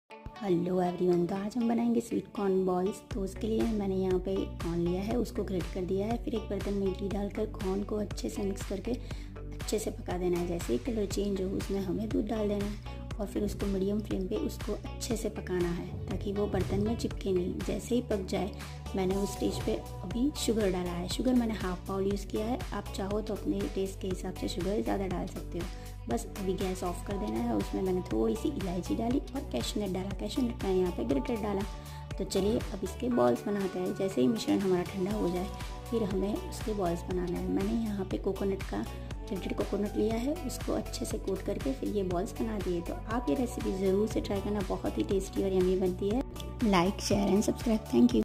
😍 Sweet Corn Balls 😋
@pinkakinu 😍😋Sweet Corn Balls 🤤

Recipes in detail
1 Sweet corn - Greated well
1 Small Bowl Milk
1/2 bowl Sugar(as per Taste)
1/2 Small spoon elichi (cardamom)
3 ,4 Peace Cashew nuts
2 Spoon Ghee
Greated Dry Coconut

First we grated corn and add into pan some ghee mix well until colour change (5 Minutes)than add milk and cook corn well into it ( Midium Flame Gas) after corn cook into milk (5 minutes)now add sugar as per Taste and mix well until jelly texar come(5 minutes) now ready gas off
Add some cardamom (elichi) add cashew nuts (grated)
Cool down mixer well
Make small peace of Balls coat into grated coconut